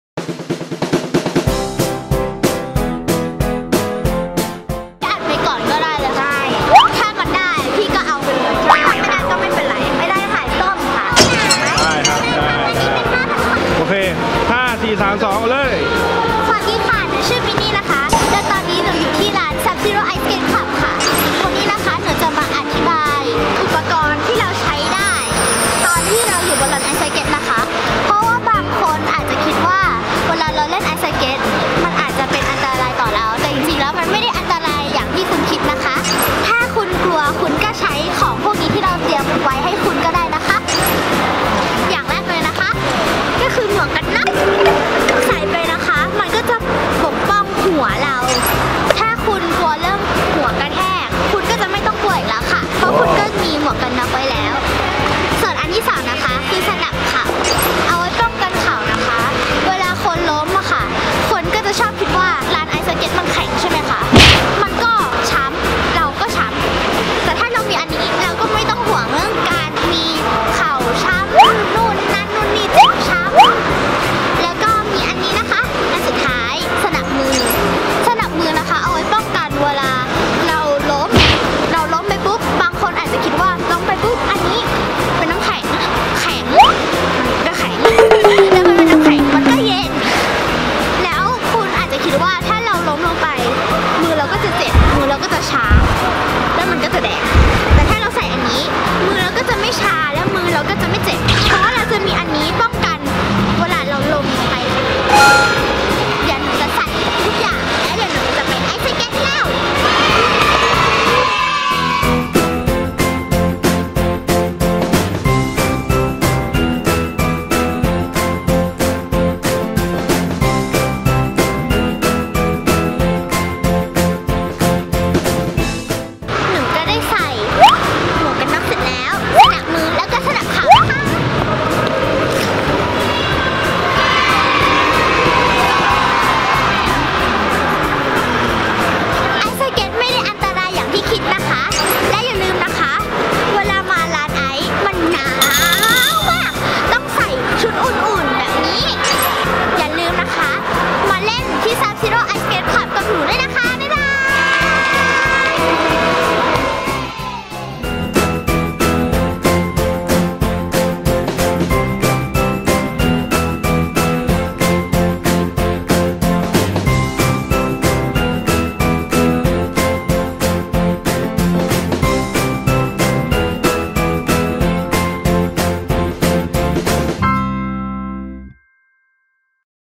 ไอซ์สเก็ต : น้องมินนี่ - นิชัญญา จากซับซีโร่ เมกาบางนา | สไตล์ Sub-Zero
น้องมินนี่ - นิชัญญา หรือมดตะนอยของลุงช้างงง นักแสดงสาวน้อย จากละครแก้วตาหวานใจ จะมาแนะนำอุปกรณ์ป้องกันยามเล่นไอซ์สเก็ตกัน!

Sub-Zero Ice Skate Club รับจัดงานเลี้ยงกีฬาสีบริษัท งานเลี้ยงลูกค้า งานการกุศล งานถ่ายโฆษณา งานเปิดตัวสินค้า